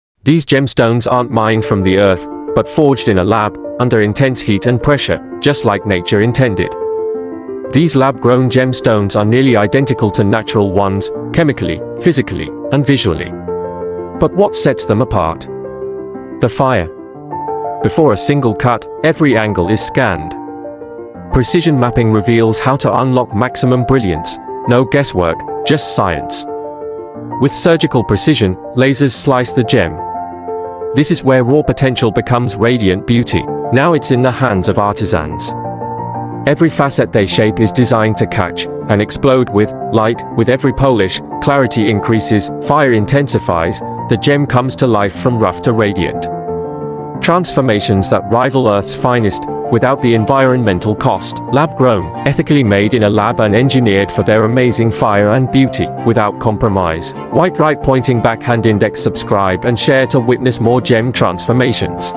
Lab Grown Diamonds That Sparkle Like Magic
Lab-Grown Diamonds That Sparkle Like Magic! These gemstones aren't mined from the Earth - but forged in a lab, under intense heat and pressure, just like nature intended. These lab-grown gemstones are nearly identical to natural ones—chemically, physically, and visually. But what sets them apart? The fire.

Original Content Owners:
Lab-grown gemstones cut & polished by @houseofsylas on YouTube & Instagram🎥📹
Scripted & Re-edited by @BlueMileWavesStudio